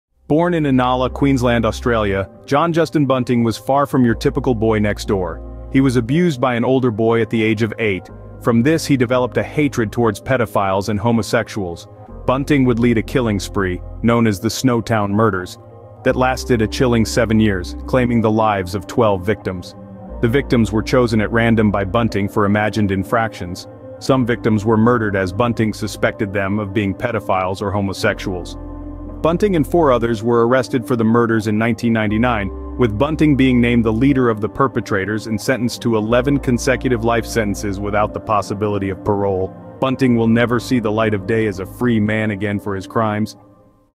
John Justin Bunting: The Snowtown Murders
A brief history on the monster that is John Justin Bunting.